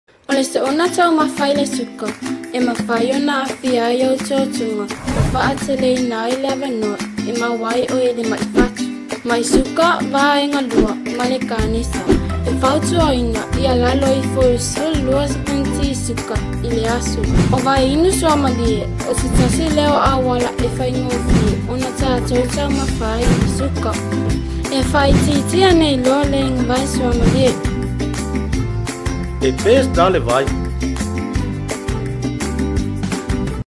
O le Soona Taumafa i le Suka ma aafiaga i lou Soifua Maloloina.
O le suka e moomia e lou tino e faatupu ai le malosi.  E moomia e lou tino I aofaiga laitiiti I aso taitasi.

O le soona taumafa I le suka e mafai ona le siia ai ou pauna ma tele ai le gao oona e pii I totoga eseese o le tino ( toxic fat)

E mafai ai ona aafia ai ou totoga ma faateleina le avanoa (risk) e  maua  ai  I le maifatu, (heart disease) mai suka vaega 2(Type 2 Diabetes) ma le kanesa(Cancer).

E fautuaina e Matagaluega Soifua Maloloina a le lalolagi (WHO) ia taumafai ia  lalo ifo o le 12 sipuniti suka i aso taitasi.

O vai inu suamalie o se tasi auala e faigofie ai ona tatou soona taumafa I le suka